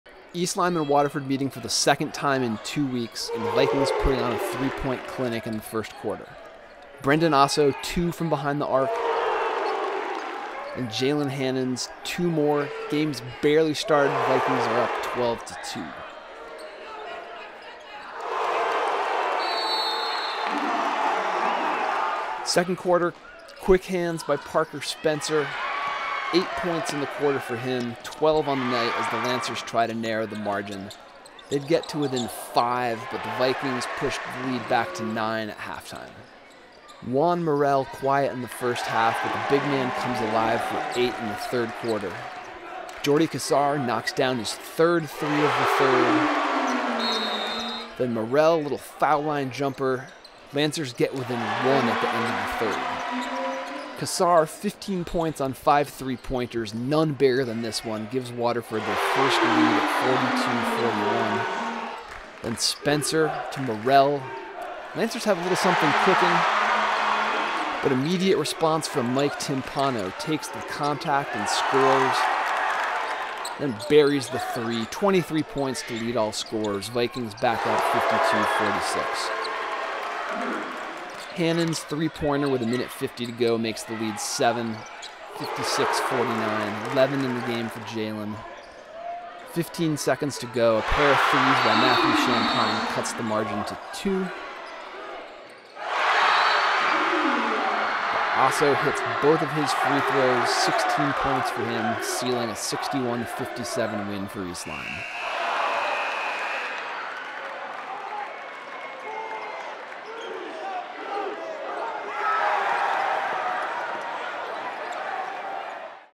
Highlights: East Lyme 61, Waterford 57
Mike Timpano 23 pts
Brendan Osso 16 pts
Jaylen Hannans 11 pts
Jordi Cassar 15 pts
Juan Morel 13 pts
Parker Spencer 12 pts